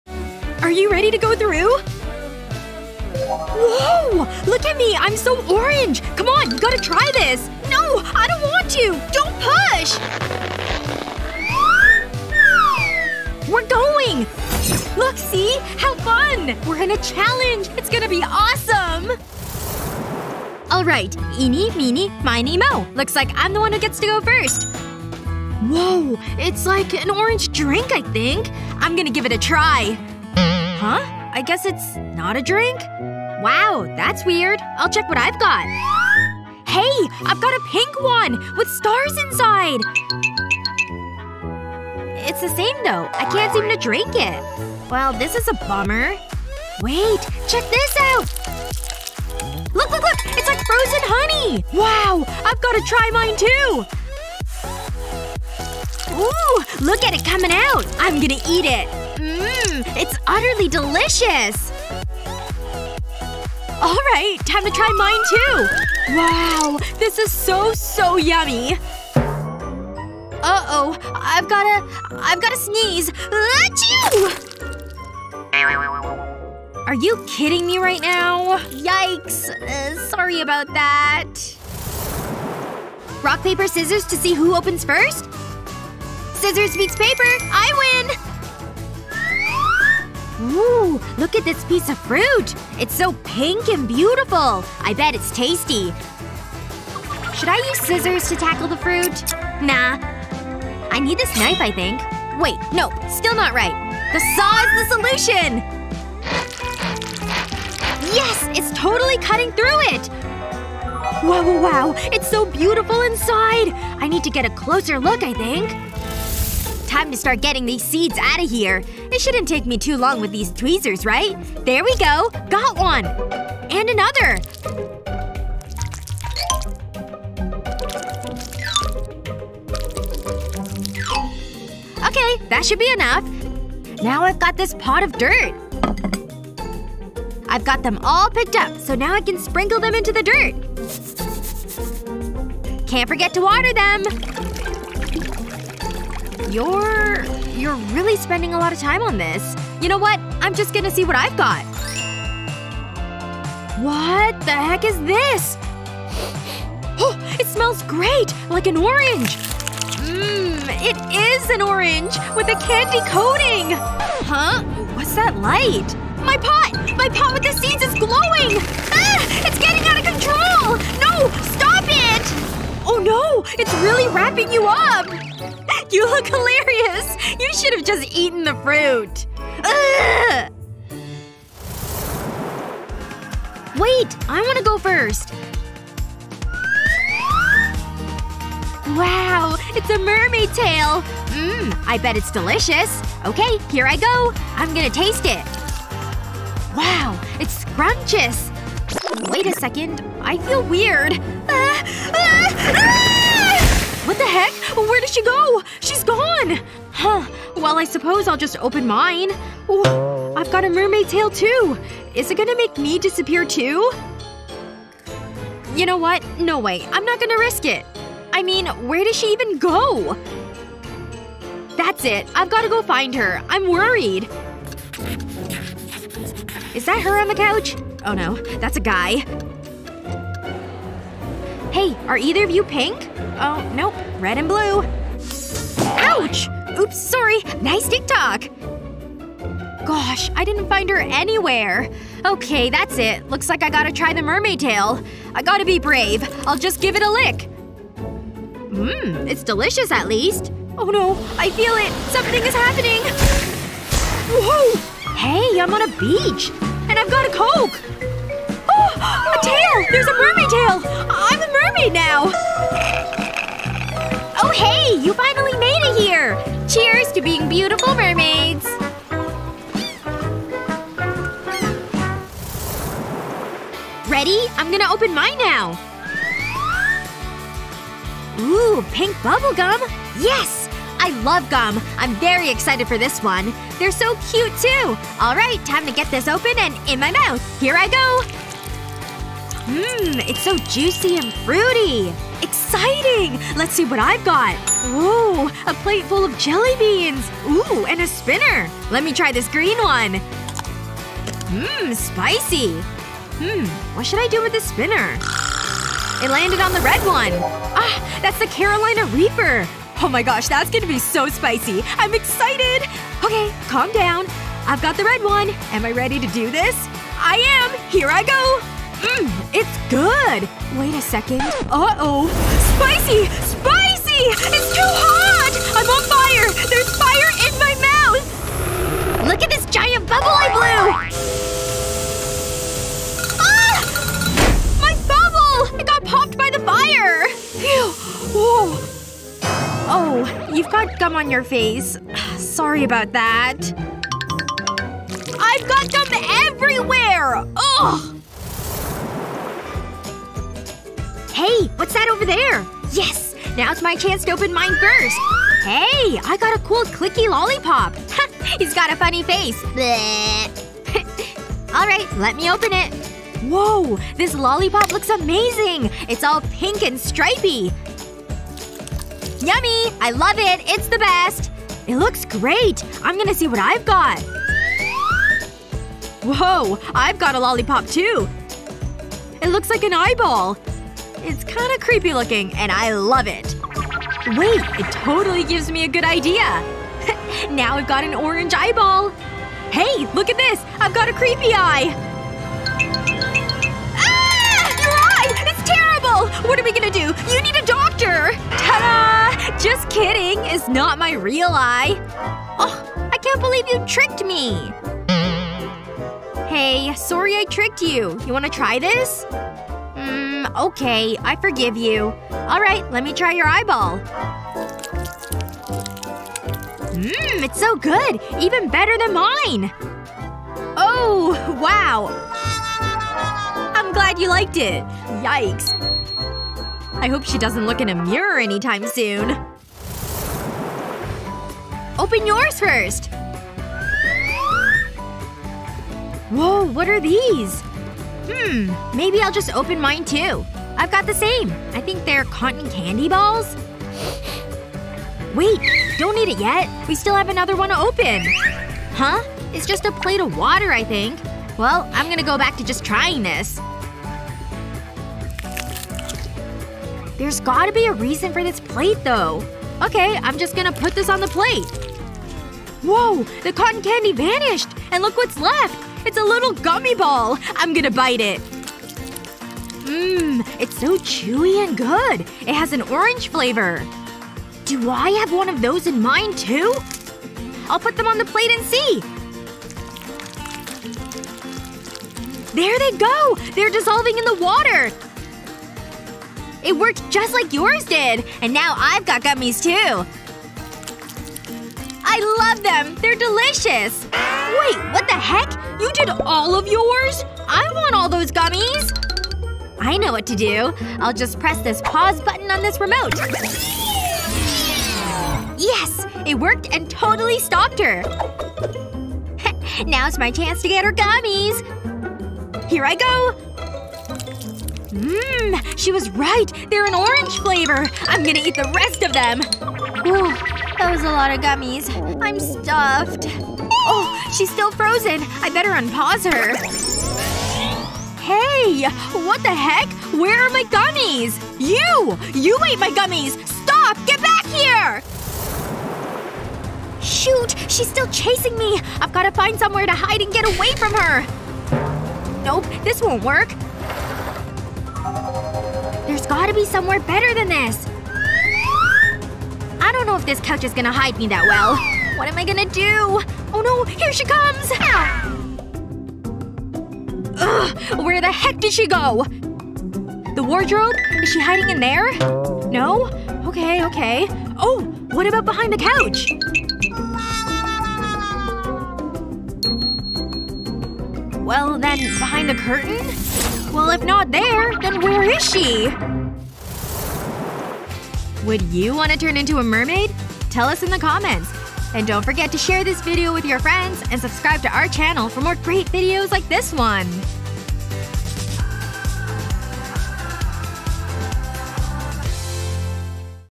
PINK VS ORANGE CHALLENGE ||Eating Only 1 Color Mermaid Food For 24 HRS! Rich VS Poor by 123 GO! FOOD
All product and company names shown in the video are trademarks™ or registered® trademarks of their respective holders. ▶️ TheSoul Music: